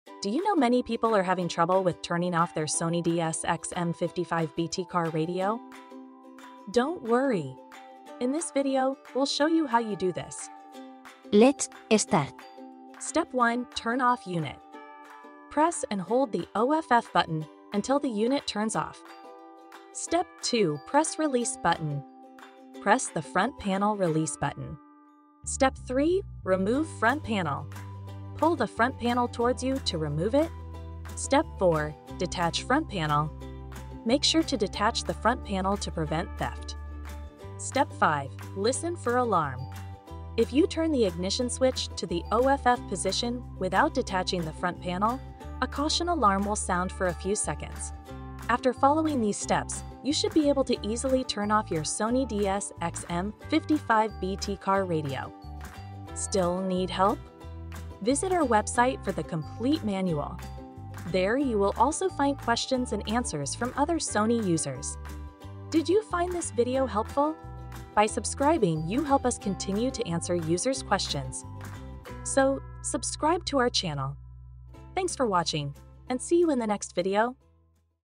How to Easily Turn Off Sony DSX-M55BT Car Radio
📺 Learn how to easily turn off your Sony DSX-M55BT car radio with this quick tutorial! Watch this video to find out how.

📖 In 5 steps:
1. Press and hold the OFF button until the unit turns off.
2. Press the front panel release button.
3. Pull the front panel towards you to remove it.
4. Make sure to detach the front panel to prevent theft.
5. If you turn the ignition switch to the OFF position without detaching the front panel, a caution alarm will sound for a few seconds.

📚💬 Manual and other questions:
On our site, you can find the complete manual for the Sony DSX-M55BT. There, you can also ask your question to other Sony DSX-M55BT users.